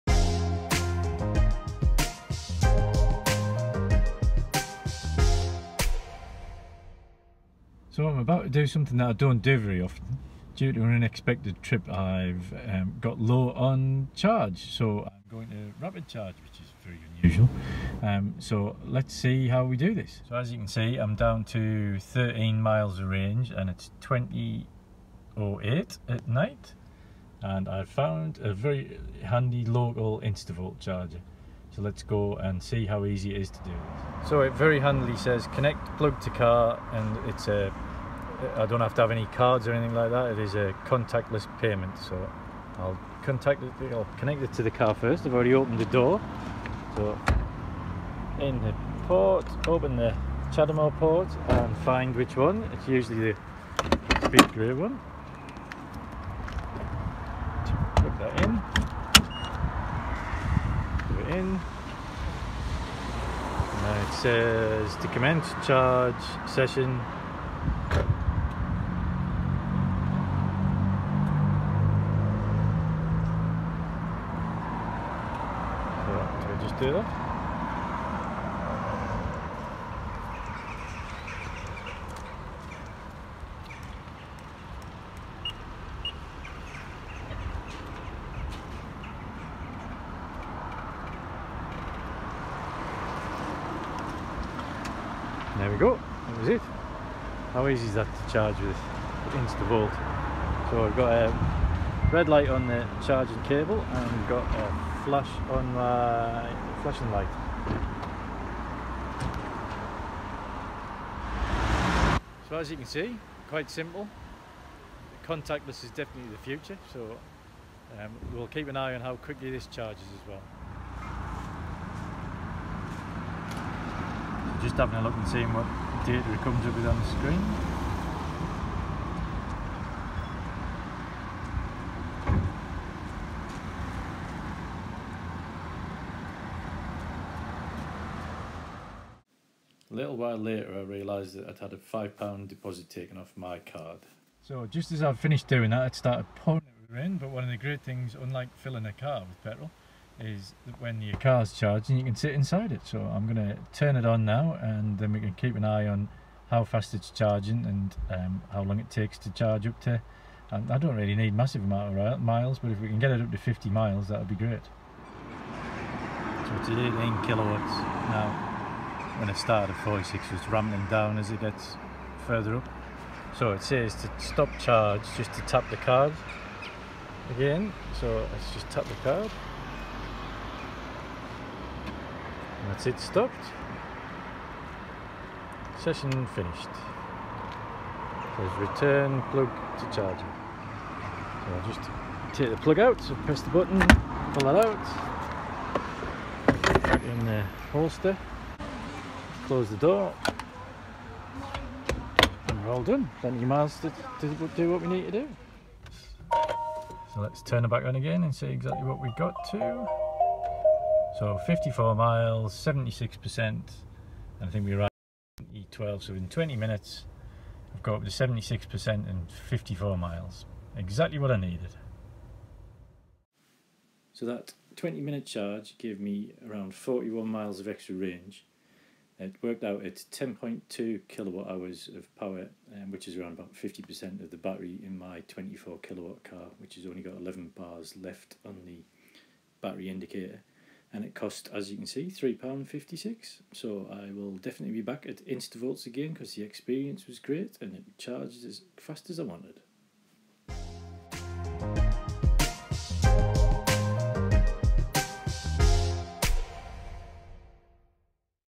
Rapid charging a Nissan Leaf at an Instavolt charger
We show you how easy it is to get a rapid charge at an instavolt rapid charger.

Nissan Leafs and electric vehicles are our speciality, check out our